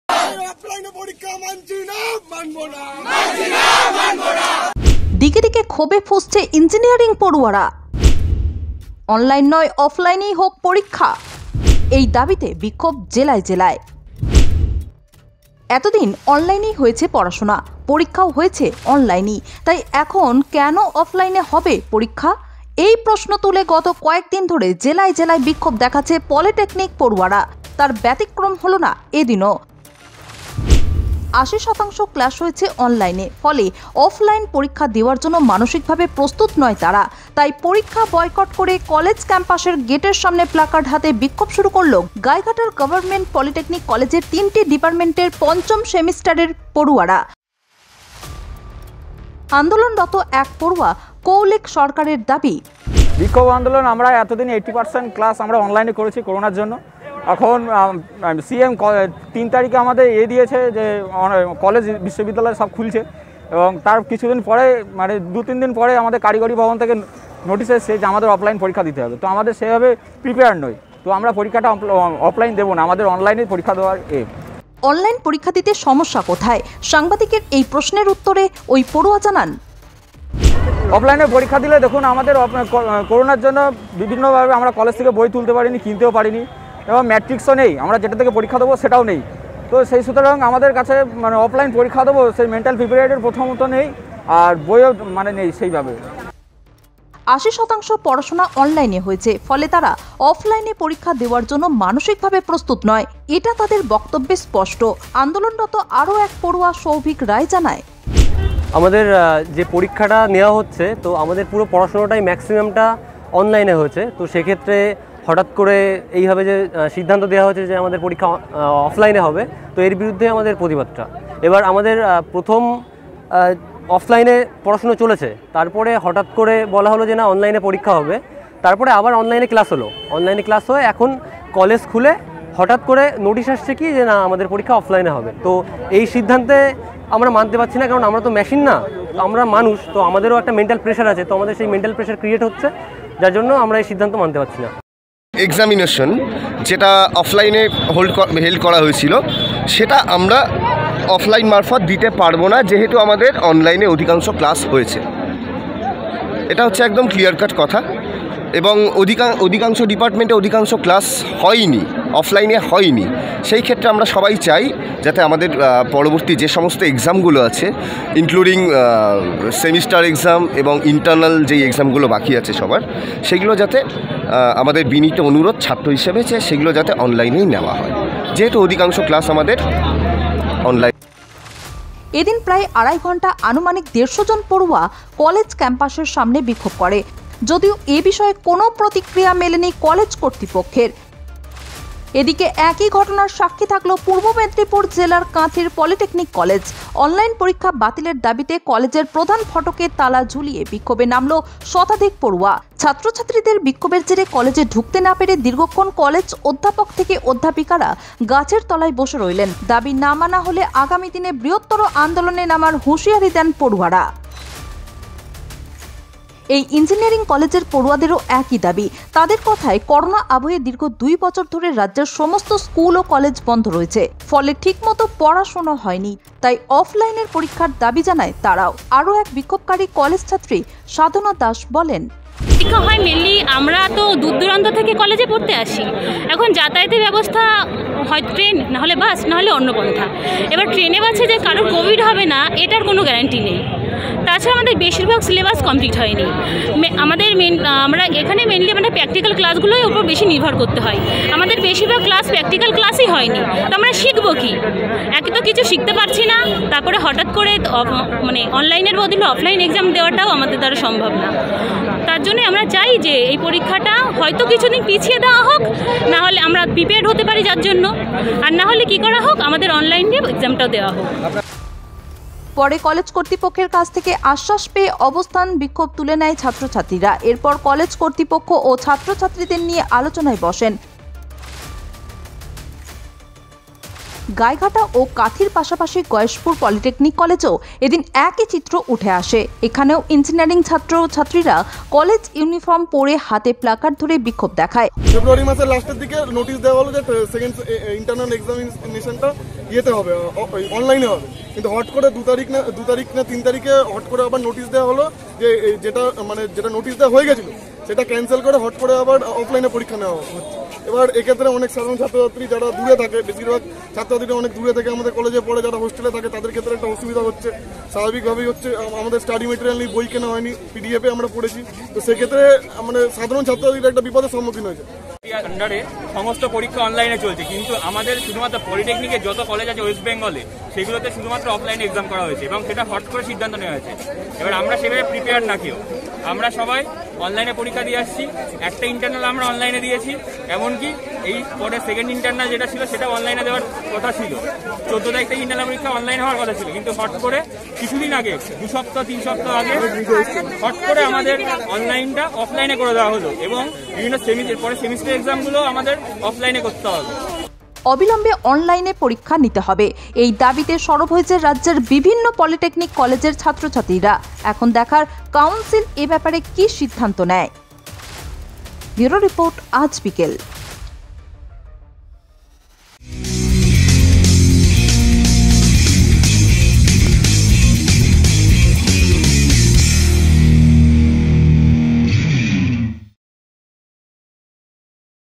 Polytechnic students across Bengal colleges protest demanding online examinations
The Polytechnic students of several colleges across Bengal districts have resorted to protests demanding for online examinations. The students claim that since classes were taken online and examinations were also held online, then why was offline examinations announced.

Fifth Semester students of three departments of the Gaighata Government Polytechnic College resorted to protests on Thursday with placards in front of the college gate campus by boycotting offline examinations. They claimed they were not mentally prepared to sit for offline examinations as 80% classes were taken online.

Watch Aaj Bikel youtube video to know the explanations given by the protesting students on what problems are they encountering in giving online examinations.

At least 150 students protested in the college campus for about 2.5 hours. The college authority is yet to issue a statement on the protest.

Similar protest was witnessed in West Midnapore district’s Kathi’s Polytechnic College. Hundreds of students locked the principal’s door and protested in demand for the cancellation of online examinations. The professors could not enter the college due to the student protests and had no option but to sit under the trees and wait.

The students warned of bigger protests in future if their demands were not met. They alleged that for the last 2 years during corona pandemic, all the schools and colleges in the state were closed and hence the studies did not take place properly. Hence, they demanded for offline examinations.

The students then lifted their protest after getting assurance from the college authority. Post this, the college administration sat with the students for discussion.

After Gaighata and Kathi, similar picture was seen in Gayeshpur Polytechnic College where students in college uniforms and placards started protesting in demand for online examinations.